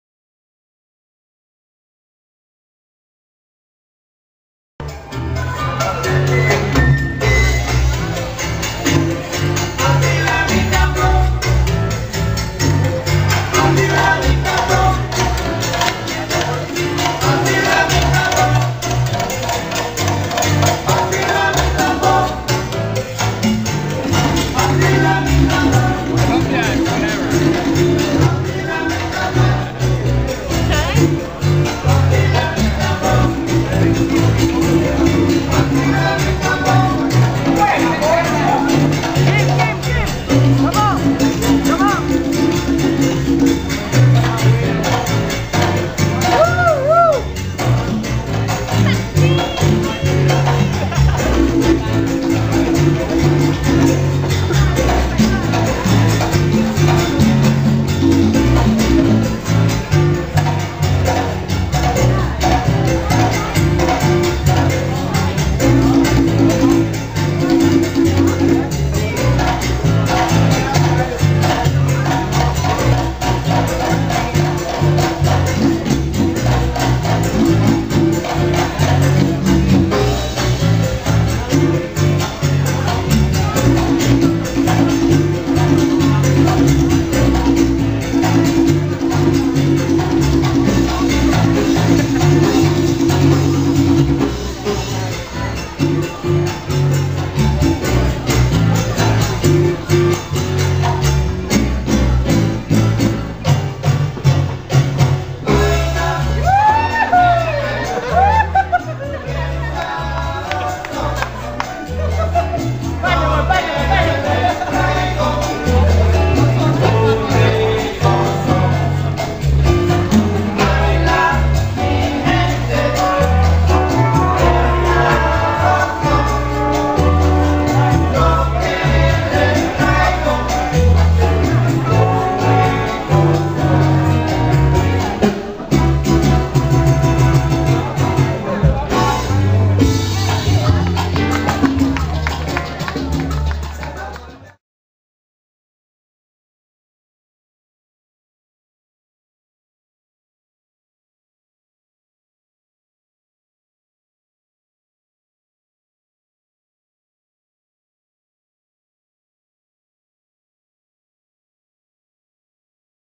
Having Fun @ Sonia's Studio
"Cha Cha" Dancers: Johana, Jose. & Houston Salseros